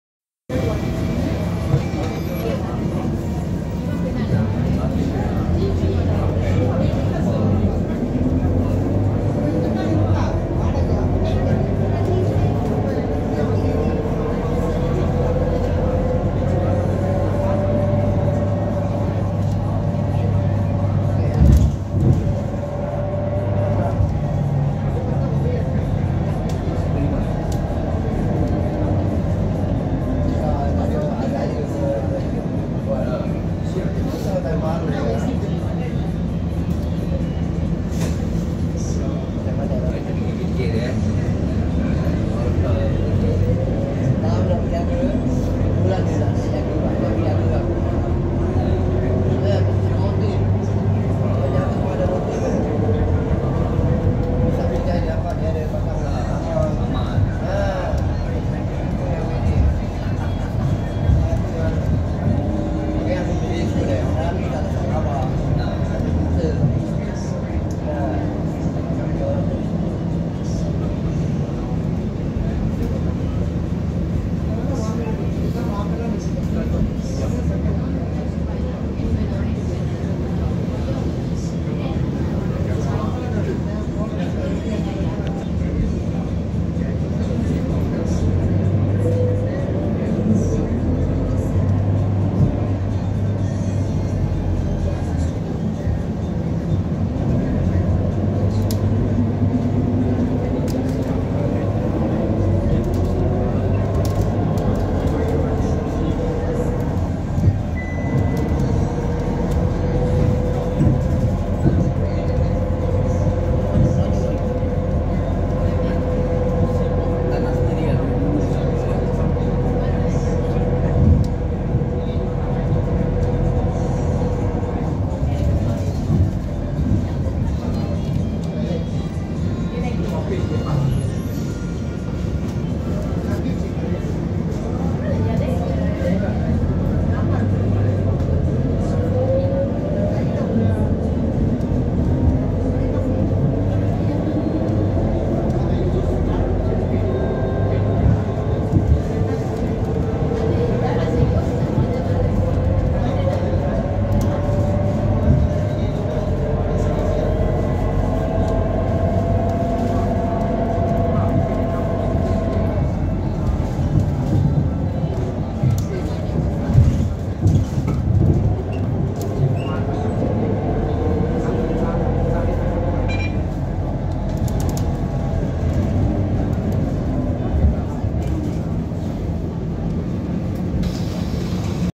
SBS8416H Service 29 Hari Raya Aidilfitri 2023
A very festive atmosphere both inside and outside the bus. Lots of people so I filmed much more discreetly.

I went to visit the Johore Battery at Cosford Rd. Did not expect so many people on the way. Silly me. Forgot about Hari Raya.

My trip:

Johore Battery is a replica of a 15 inch artillery gun used to defend Singapore during WW2, it has since closed down based on the lack of recent information and padlock gates plus general signs of abandonment.  No surprise since it is inconvenient to get to. Walking up Cosford Rd, I saw a Prison service building and Aetos K9 unit. There is no footpath so I walked along the road to the site. No shelter and no proper place to park any vehicles. Can see why it would close.